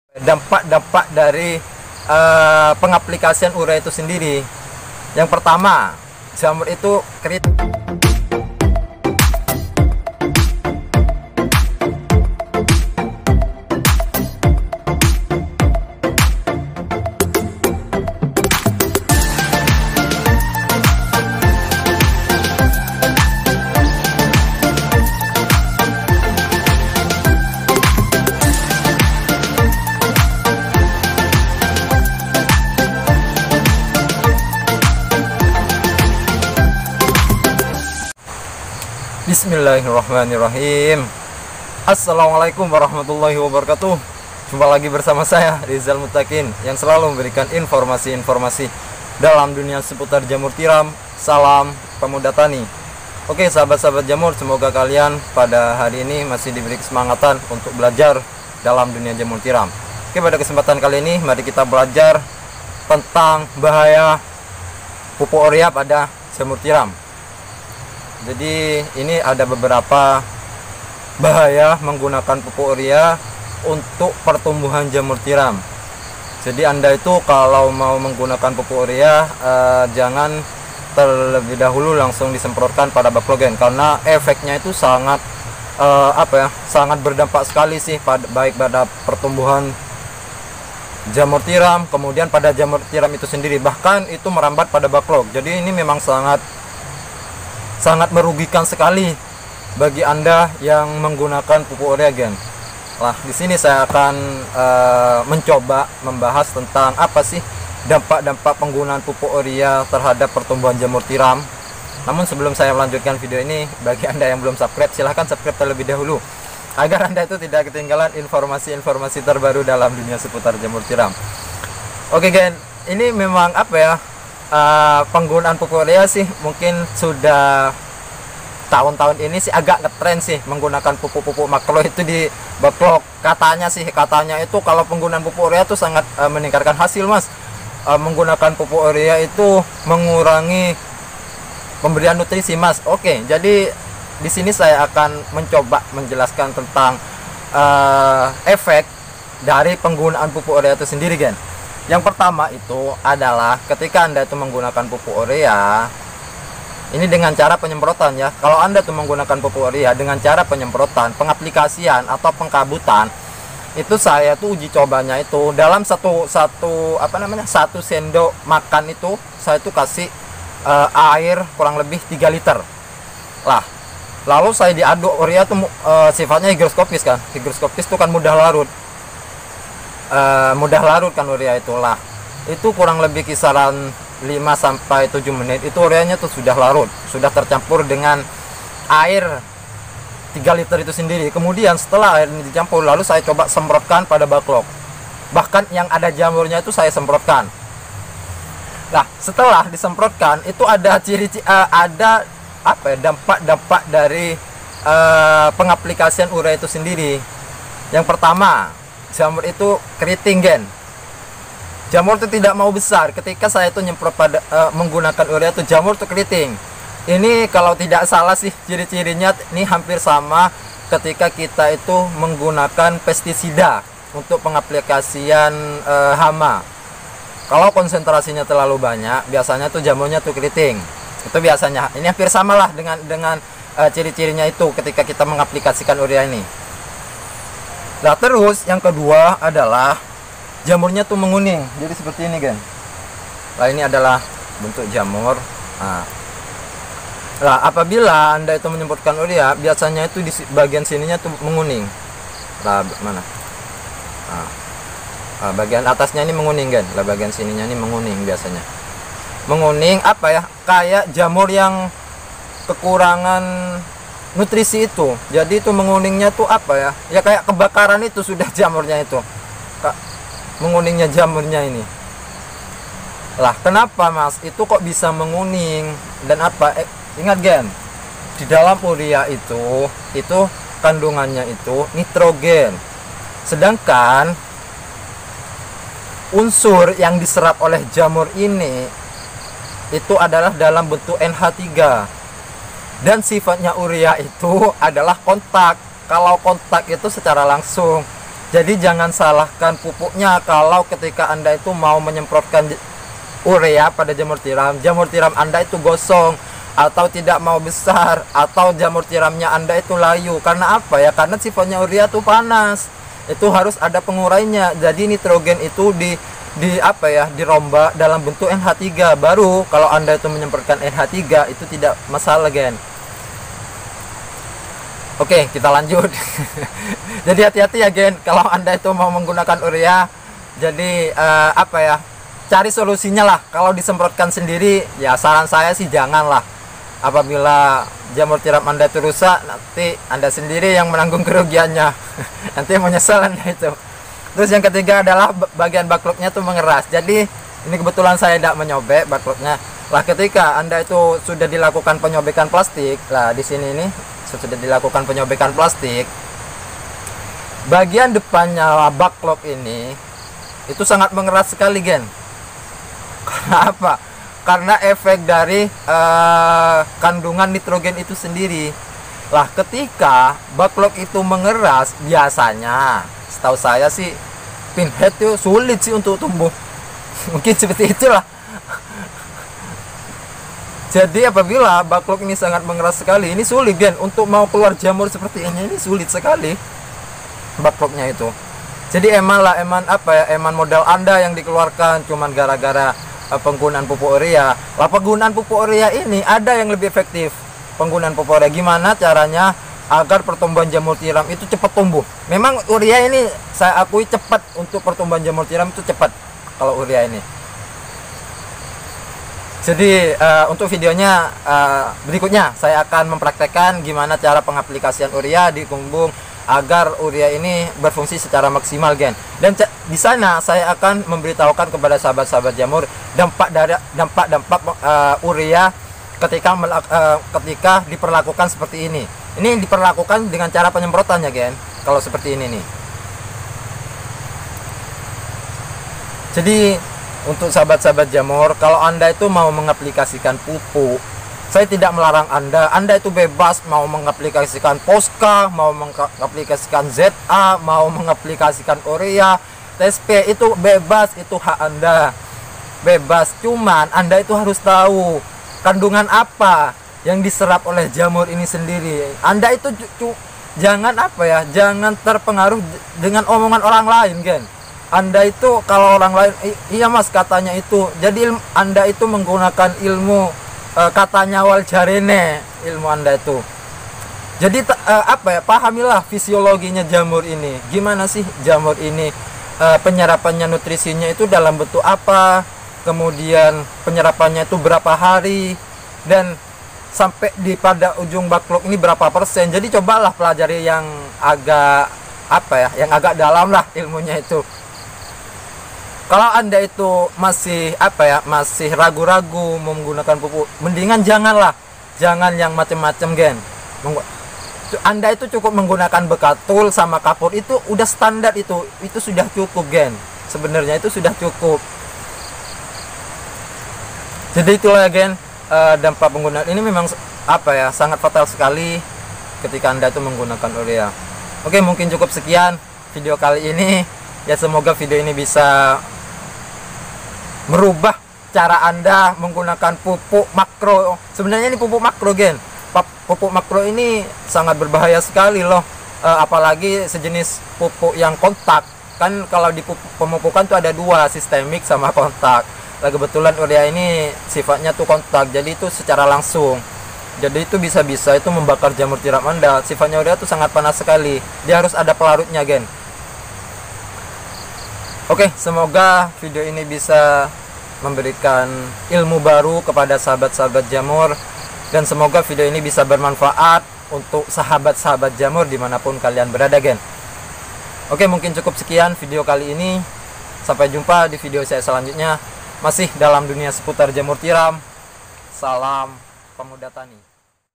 PENGARUH PUPUK UREA TERHADAP JAMUR TIRAM
pupuk urea merupakan pupuk yang biasa di gunakan bagi tanaman. Pupuk urea memiliki sifat kontak yang memiliki kandungan nitrogen.
 Pupuk Yang Di serap Oleh Jamur Yaitu Dalam Bentuk NH3 bukan dalam bentuk Nitrogen, Apa bila Kandungan Pupuk Urea tersentuh Pada Jamur Tiram Maka Jamur tersebut Akan Menjadi Rusak